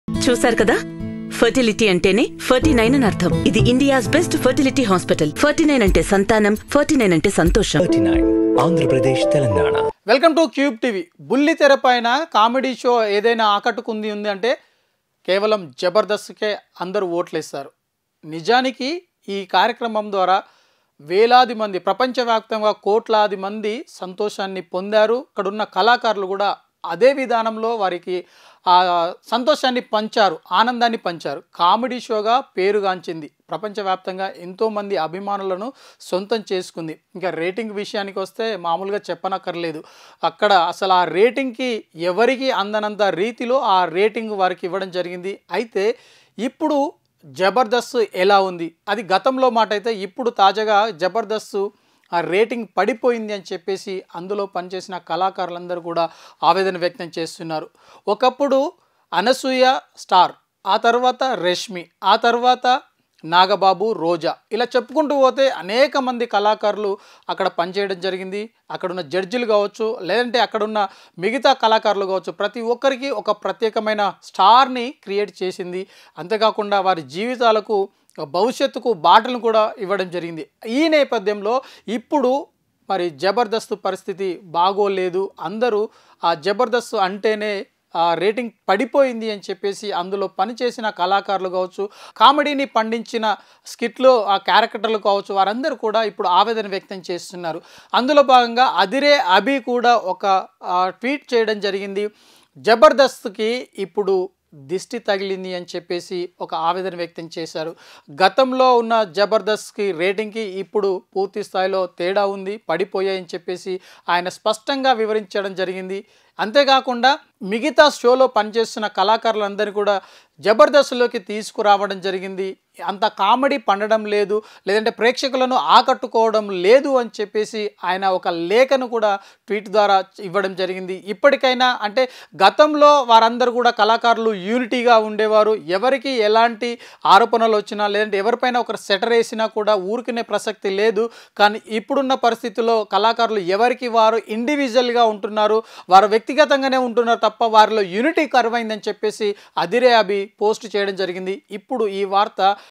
Adhire Abhi Reveals Shocking Facts About Jabardasth Show Low Rating | Aadi | Sudheer | Daily Culture
Adhire Abhi Reveals Shocking Facts About Jabardasth Show Low Rating | Aadi | Sudheer